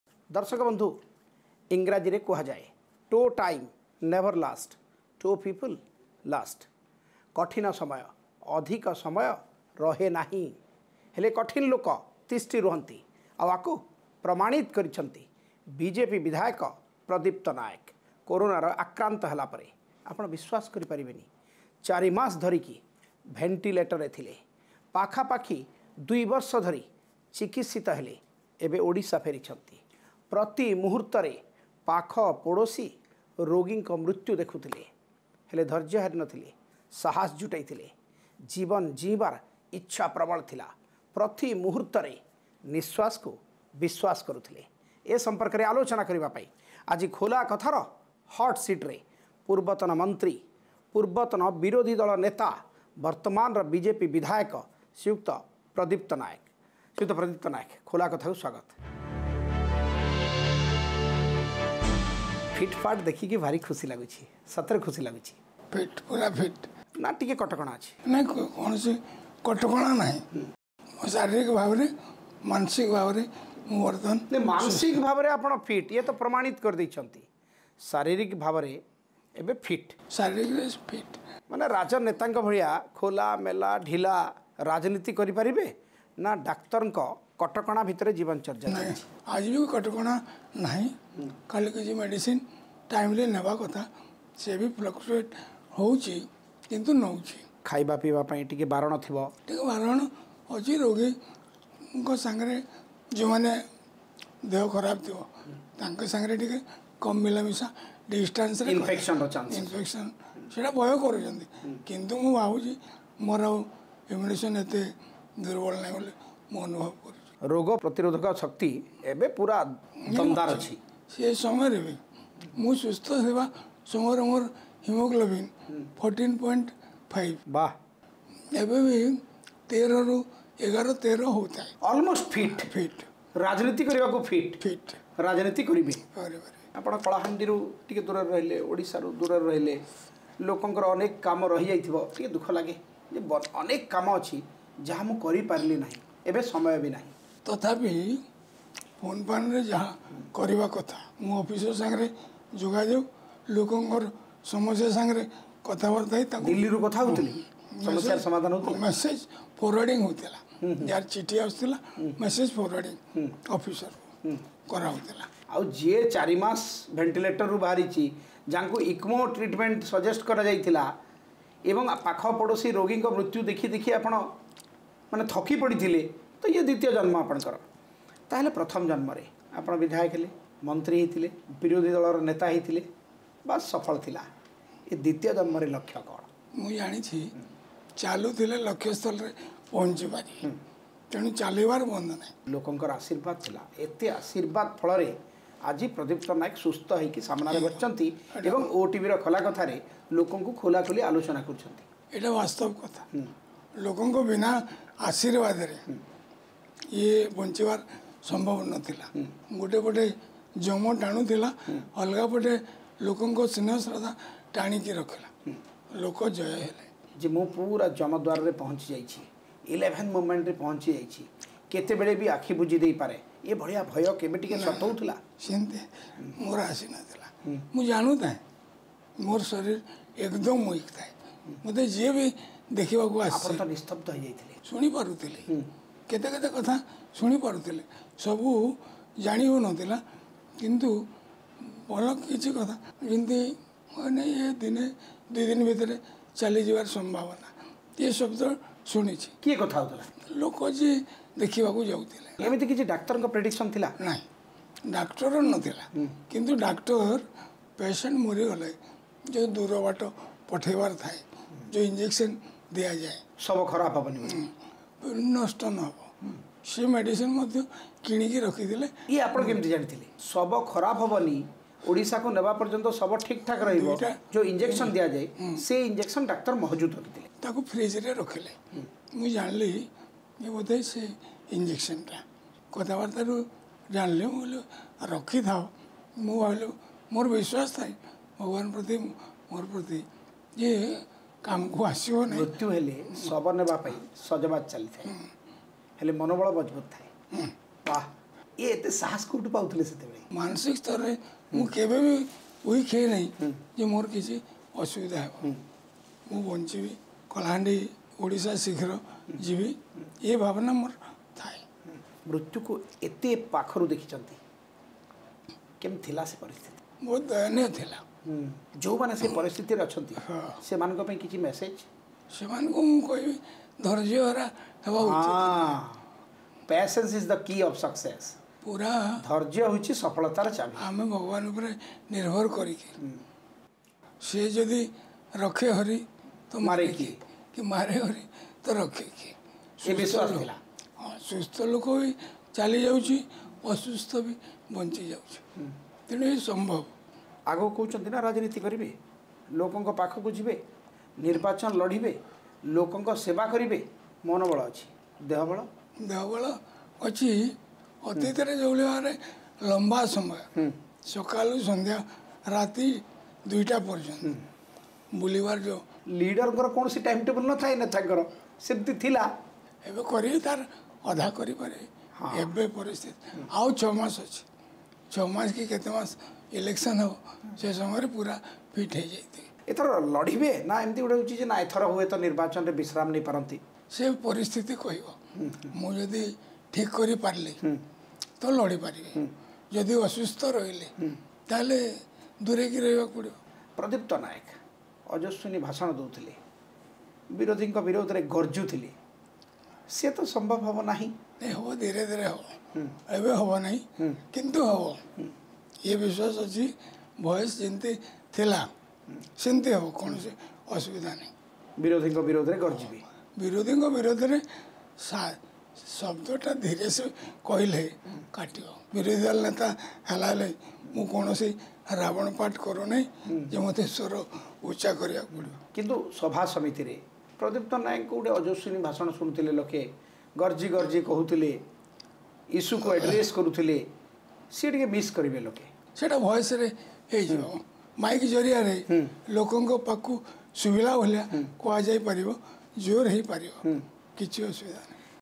Khola Katha with BJP leader Pradipta Naik, who return home after undergoing treatment for 26 months
Khola Katha with BJP leader Pradipta Nayak, who return home after undergoing treatment for 26 months